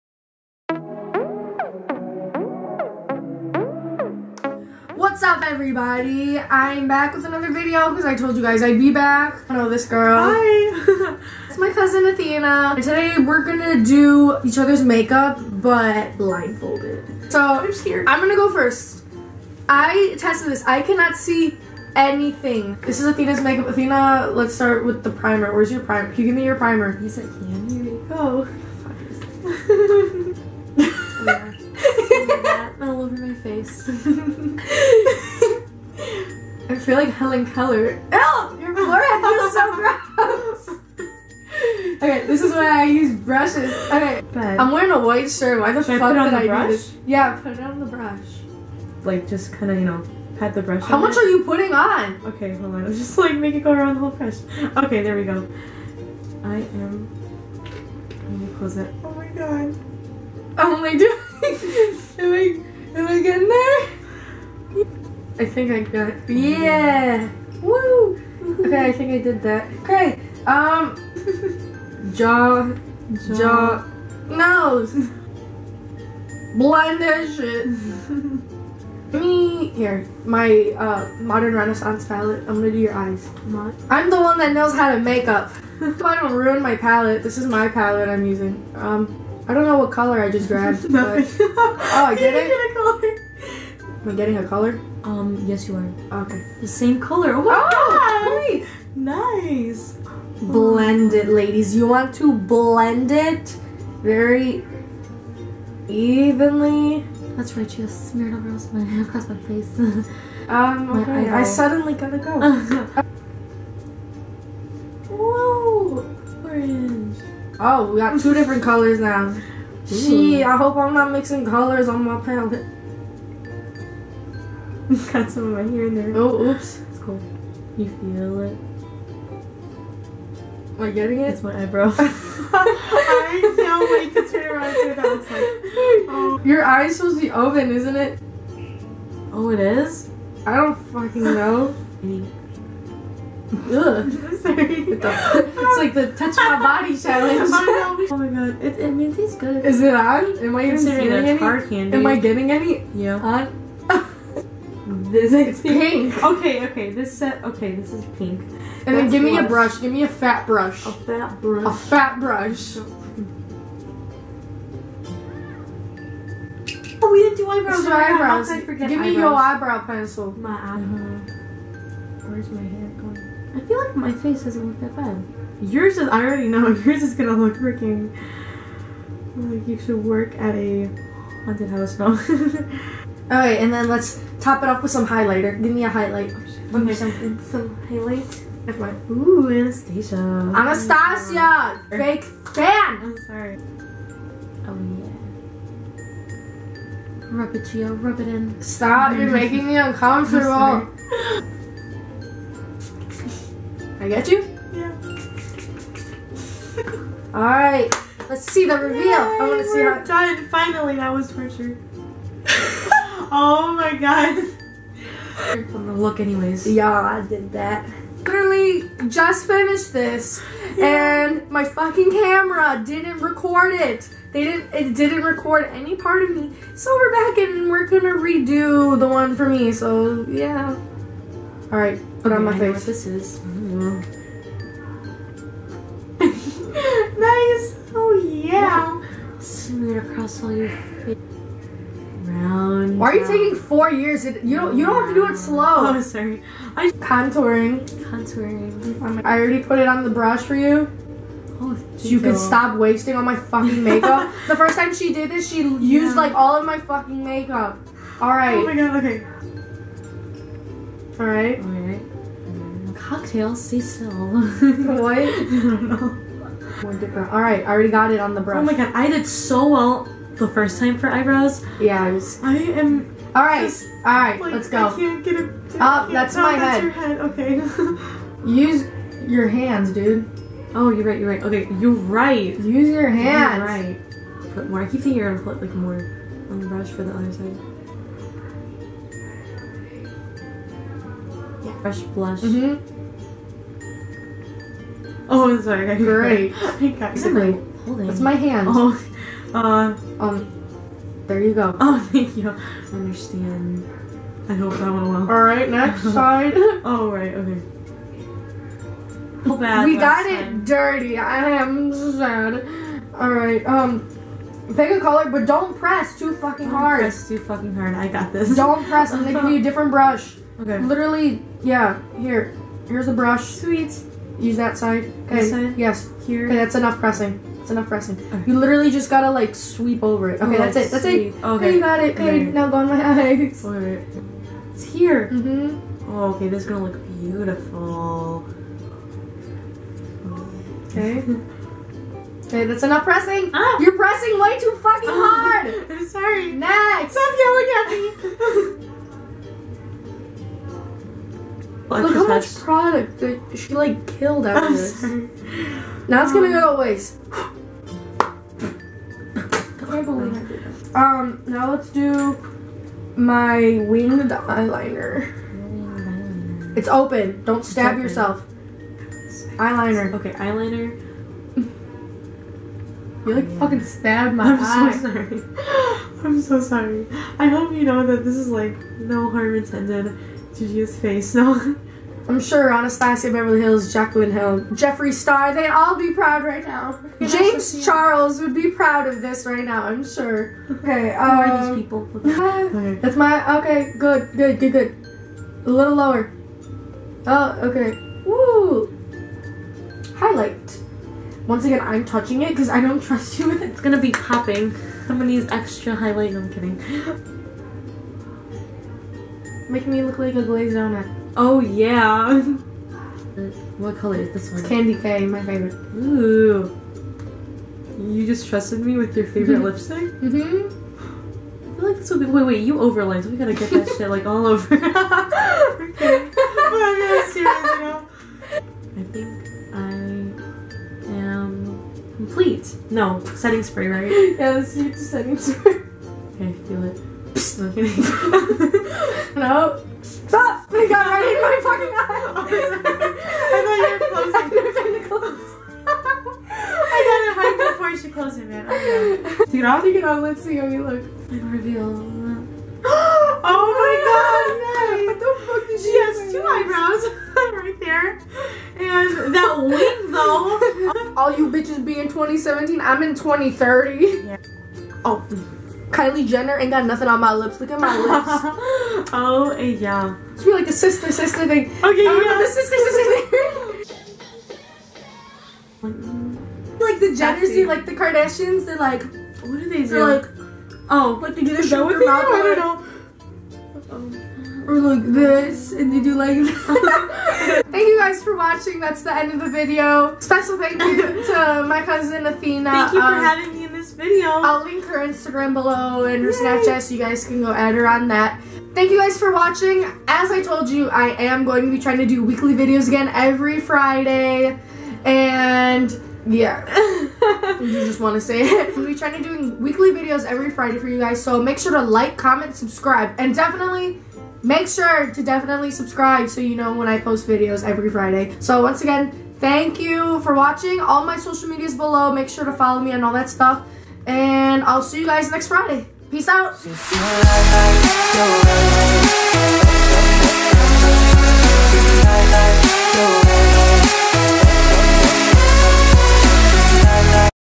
BLINDFOLDED MAKEUP CHALLENGE *COMPLETE FAIL* | imactuallygia
HIIIIIIIIIIIIIIIIII! back with another video for you guys, as promised!

Here's her Instagram and Snapchat, so make sure to hit her up and follow her on those!

@ athina.paria

@ gialovesbieber